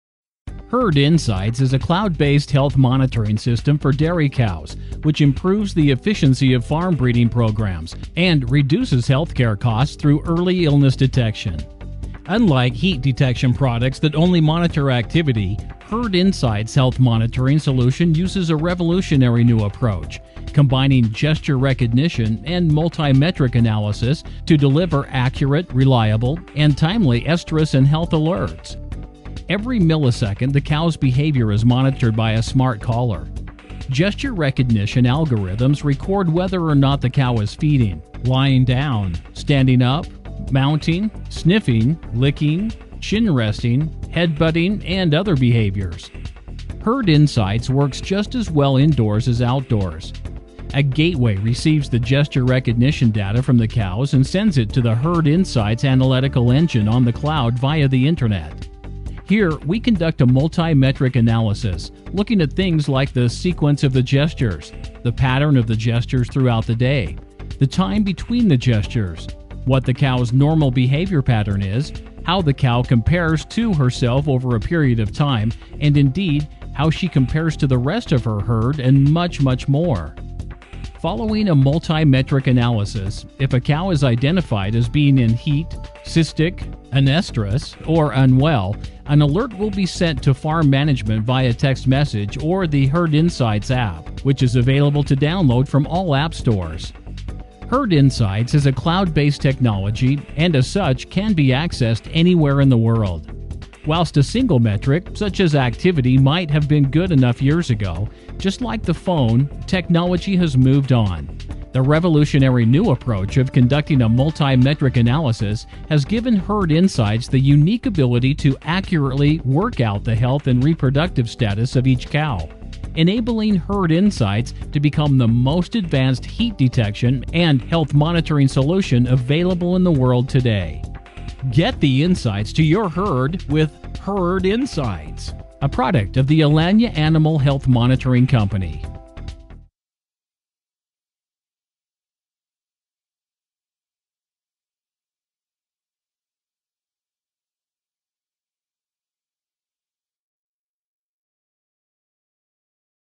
HerdInsights Heat Detection and Health Monitoring of Dairy Cows
A revolutionary cow monitoring system which conducts a multi metric analysis to determine optimum insemination time, cow health status, lameness and heat stress